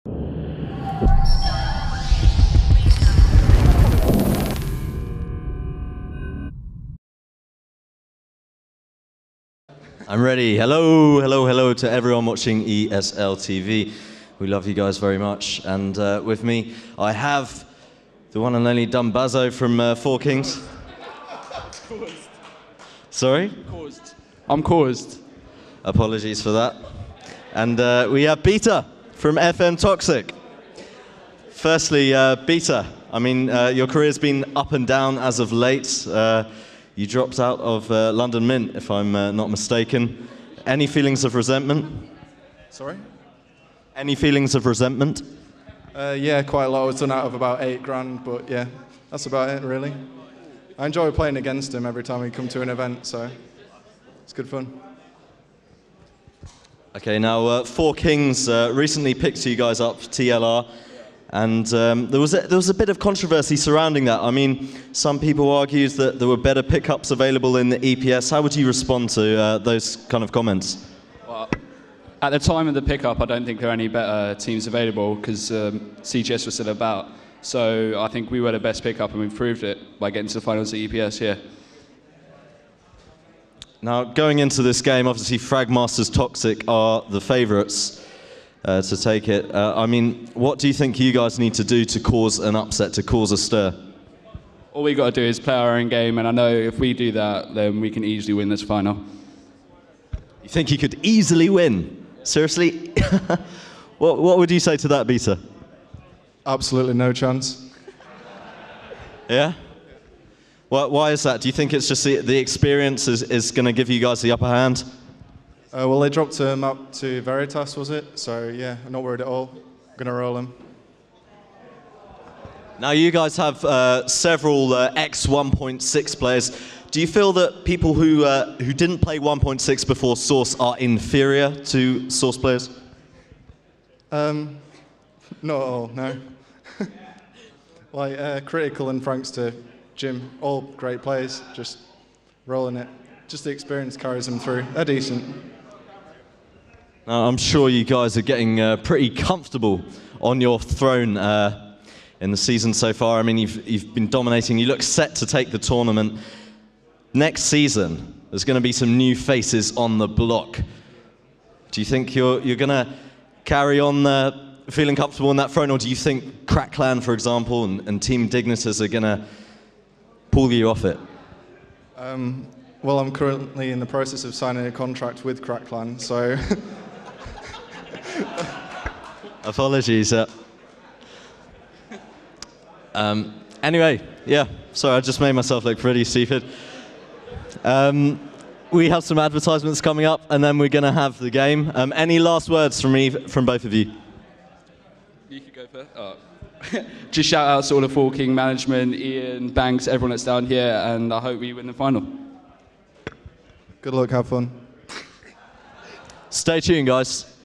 Pre match Interview with Cauzed and Beta - EPS Season 1 finals
Pre match Interview with Cauzed and Beta that took place moments before the grand final between FM Toxic and 4kings at the EPSUK Season 1 finals